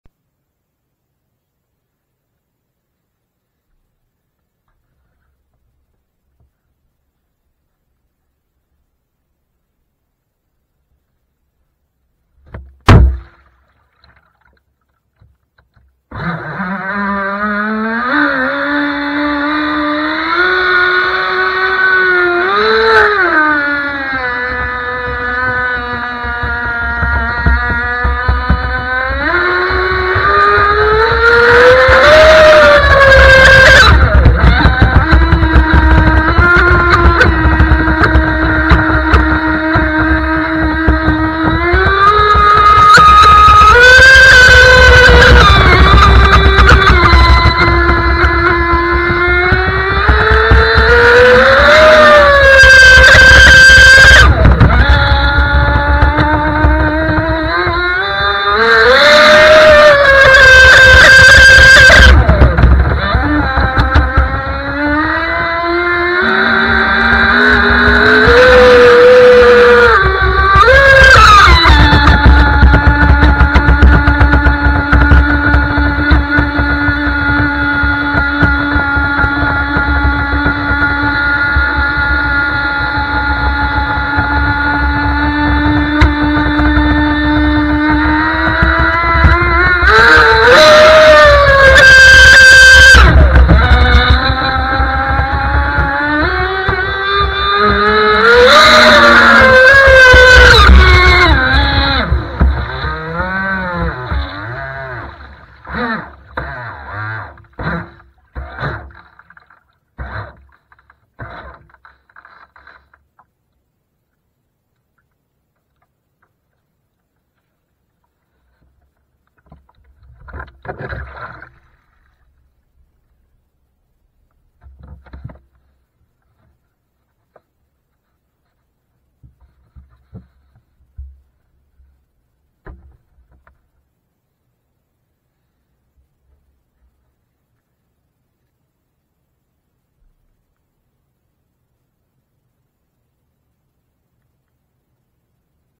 Sonicwake 36 50 mph run with action cam
Just having a little fun with the action cam mounted atop the Sonicwake. Not as fast as I was hoping to achieve, but 50 mph is ok for now. Hopefully be in the 60's soon!